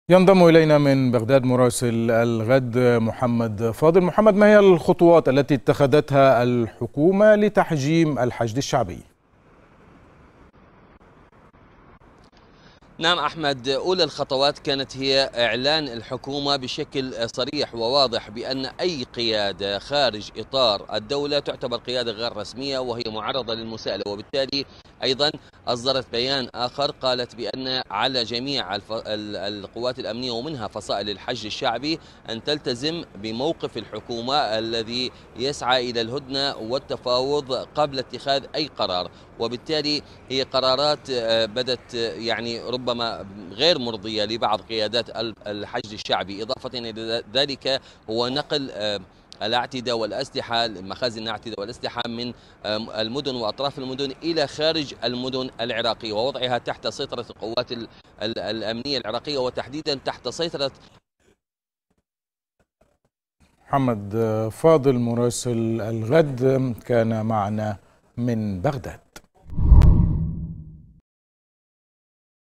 هل تنجح الحكومة العراقية في تحجيم الفصائل المسلحة؟
شهدت الساحة العراقية تطورات كبيرة عقب الضربات الأخيرة التي نفذتها طائرات مسيرة على مواقع للحشد الشعبي الذي اتهم إسرائيل بالوقوف خلفها.

ومن بين هذه التطورات خلافات وانشقاقات داخل قيادات الحشد الشعبي وإعلان الحكومة_العراقية عزمها تحجيم عمل الفصائل المسلحة.

المزيد من التفاصيل يتحدث مراسل قناة الغد من بغداد، محمد فاضل، عبر النشرة الإخبارية لقناة الغد.